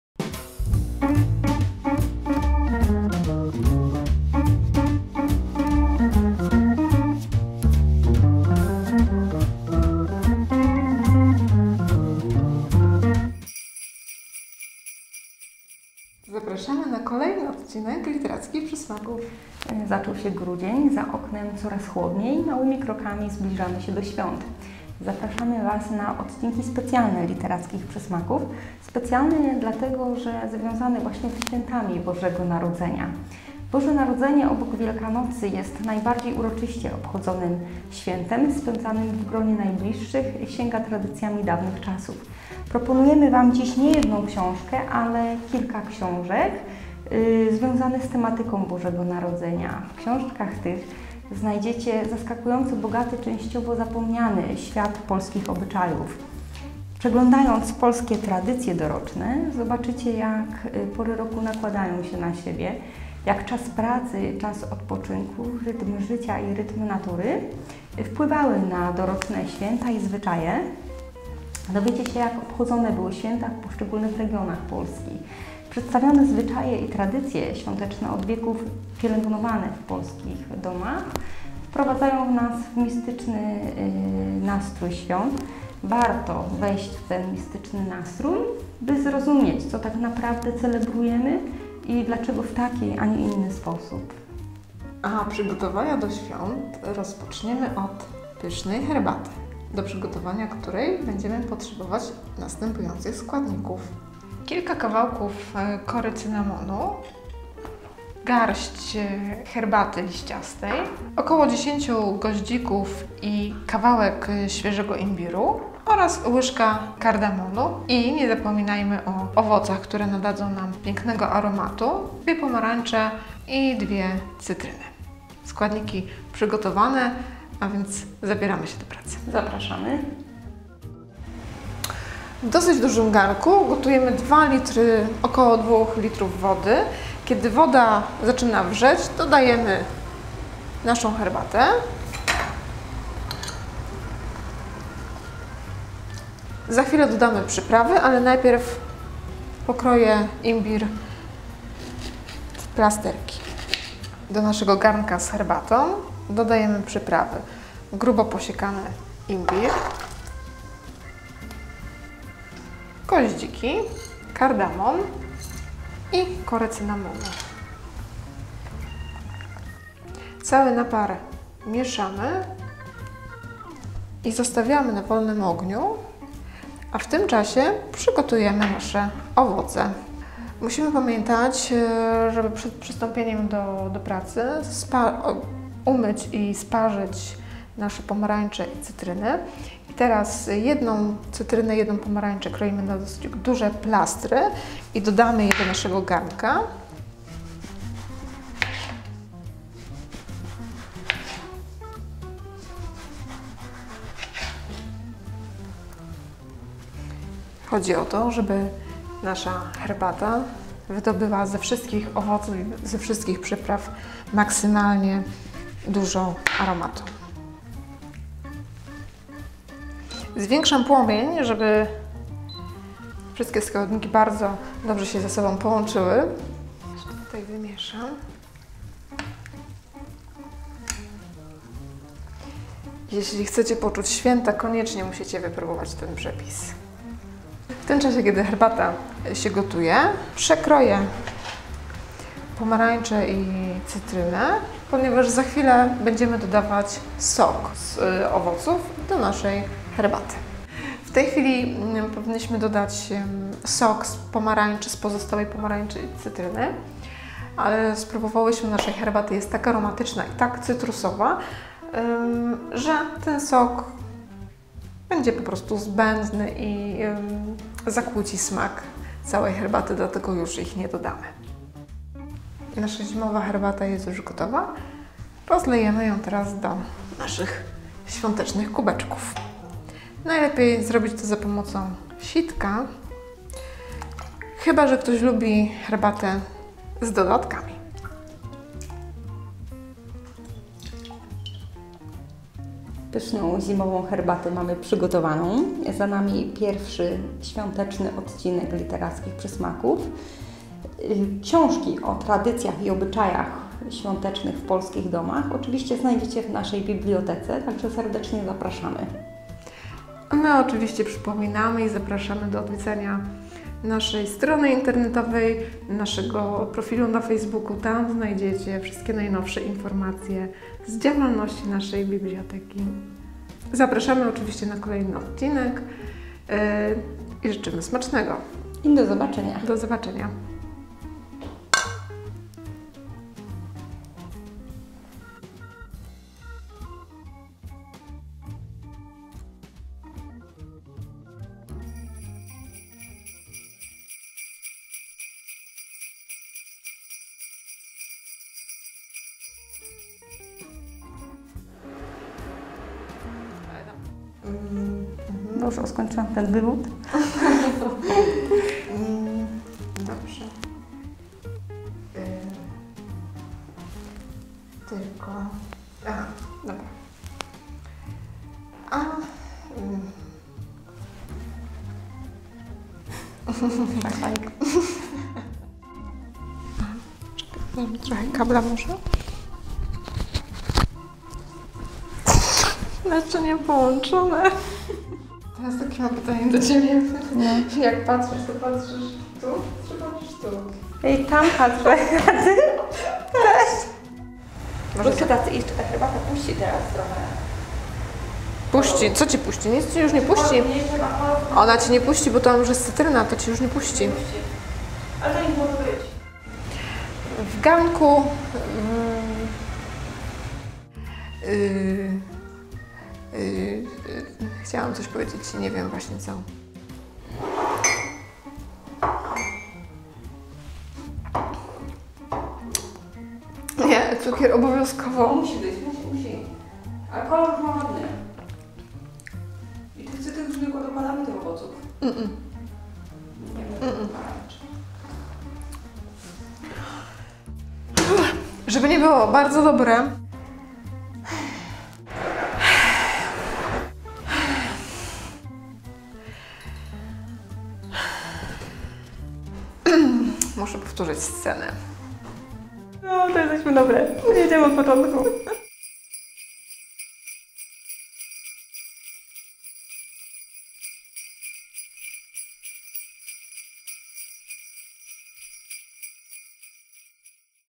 Literackie przysmaki - odc. 20 "Zimowa herbata"
Zaczął się grudzień, Święta coraz bliżej... "Literackie przysmaki" wraz z czytelnikami kościańskiej biblioteki przez najbliższe tygodnie będą się przygotowywać do Świąt Bożego Narodzenia. Oczywiście, w towarzystwie książek ze zbiorów MBP. W dzisiejszym odcinku do przygotowania zimowej herbaty zainspirowały nas książki o tradycji Bożego Narodzenia.

Potrzebne składniki:
- garść liściastej herbaty yunnan,
- kilka kawałków kory cynamonu,
- 10 goździków,
- łyżka ziaren kardamonu (zielonego),
- 6 cm obranego imbiru,
- 2 cytryny i 2 pomarańcze (obrane i sparzone),
- do posłodzenia - cukier lub miód